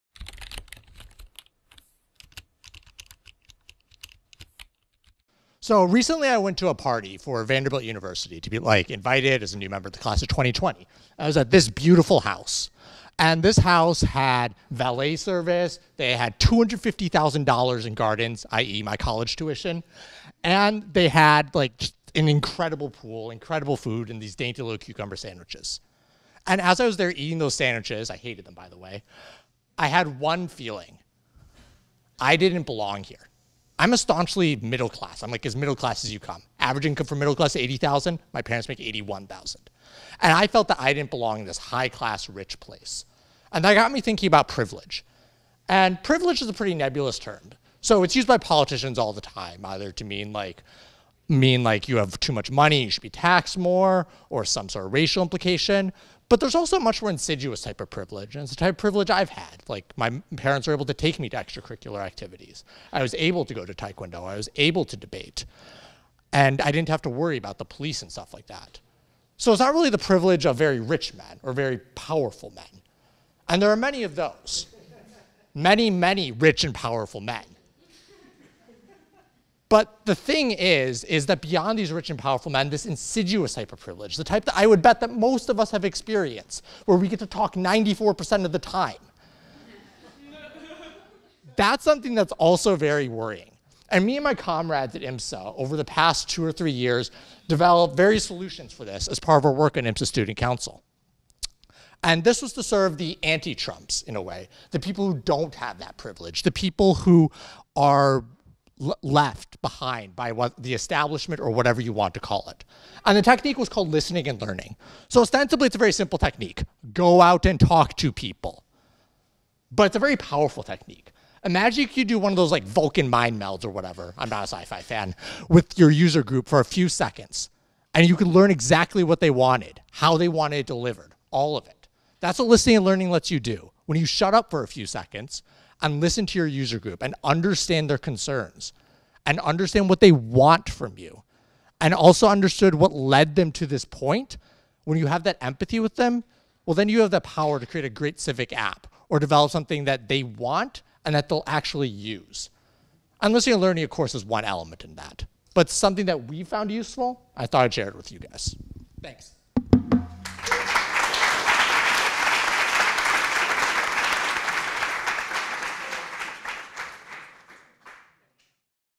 Benjamin Cooper: Learning to Listen
All of us have different values but broadly speaking, we want to make our community better for _everyone_. The problem: the people building this tech probably live very different lives that the majority of people in our community. In that context, listening to the community and learning from it to deploy our skills in the right direction is important.

On July 19th 2016, we heard from 9 members of Chi Hack Night about what they’ve done at or learned from our group.